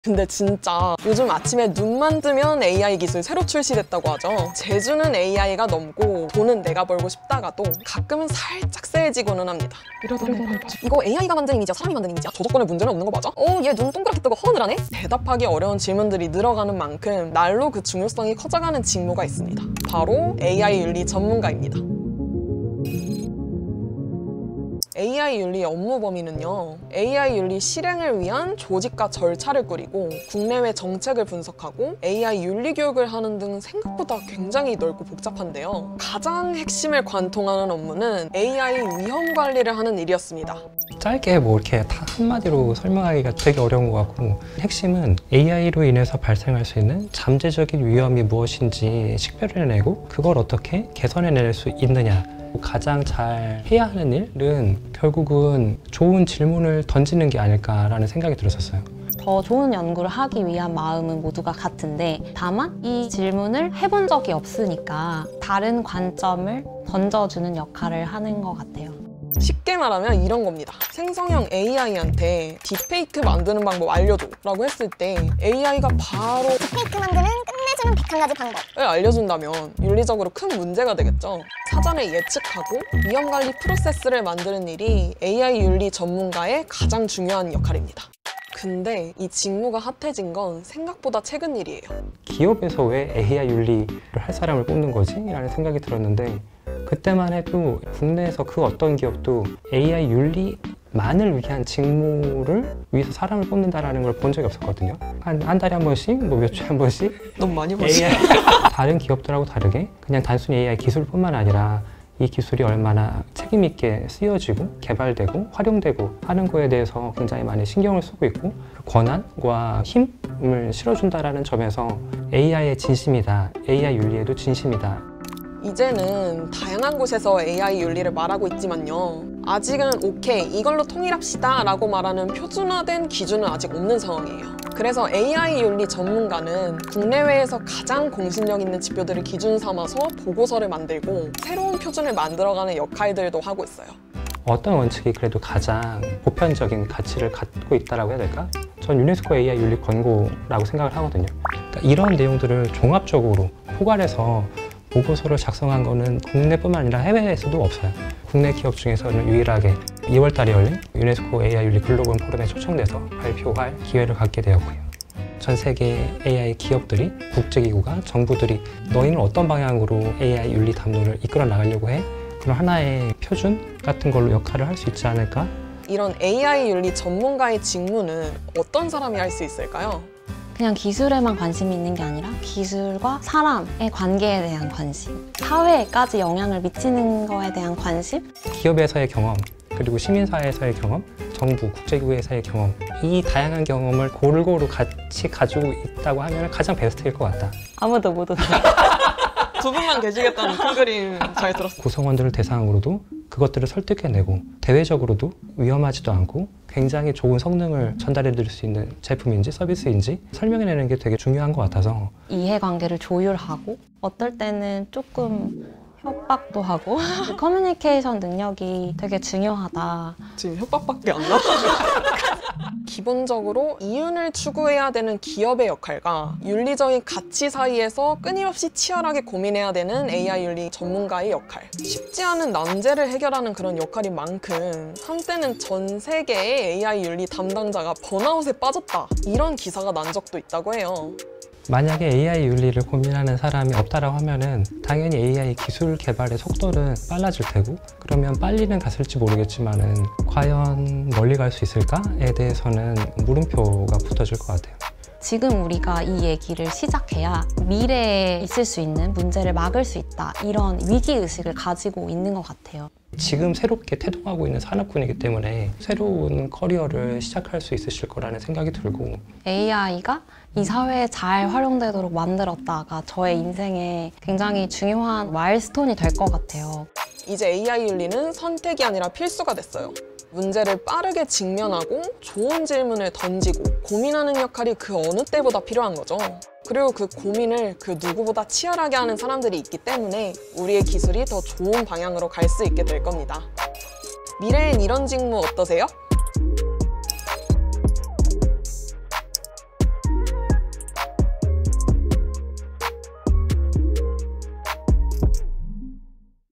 날뛰는 AI 문제 잡으러 온 AI 윤리 전문가ㅣ미래직무탐구 EP.1
하루가 다르게 발전하는 AI 기술! 빠르게 발전한 만큼, AI 사용의 윤리적 문제 또한 폭증하고 있는데요. 어떻게 써야 AI를 '올바르게' 사용할 수 있을까요? 이런 기준은 대체 누가 정하는 걸까요?

오늘의 주인공은 혼돈의 AI 시대의 기준을 만들어나가는, 'AI 윤리 전문가' 김명신 님과 안소영 님입니다. AI 윤리 전문가는 AI 기술을 통해 발생할 수 있는 잠재적 위험을 식별하고 개선하며, 기술과 윤리의 가교역할을 한다는데요.

AI 윤리의 새로운 표준을 만들어 나가는 'AI 윤리 전문가'의 이야기. 지금 영상에서 확인하세요!

00:00 혼돈의 AI 시대, 어떻게 해야하지?
00:23: AI 윤리의 '기준'을 세운다는 'AI 윤리 연구원' 🌟
01:28: AI 윤리 전문가가 요즘 HOT🔥 하다고??
02:25: 유네스코가 선택한✨ LG의 AI 윤리 연구!!
02:58: AI 윤리 전문가의 필수 덕목 = 소통?🤔